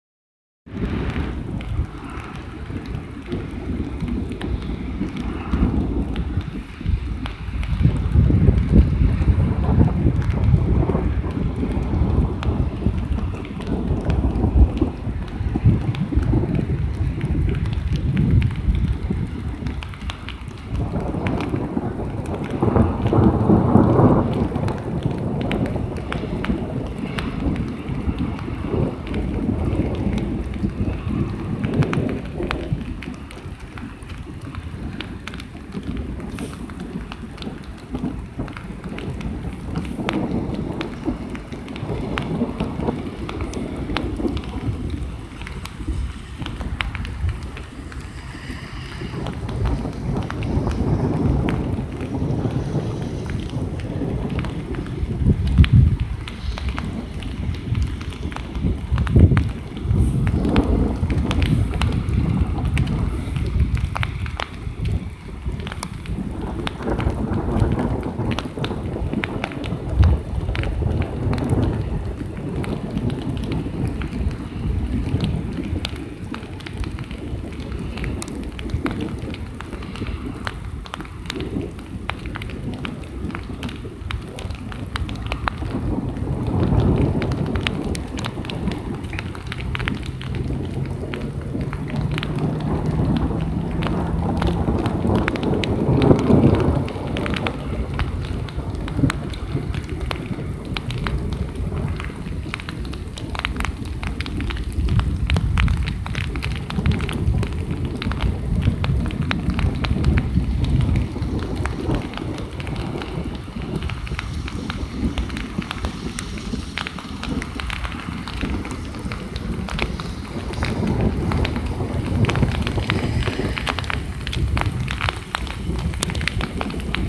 Tormenta (Relampagos y Descargas Electricas) en la Oscuridad
Una noche oscura y fria de tempestad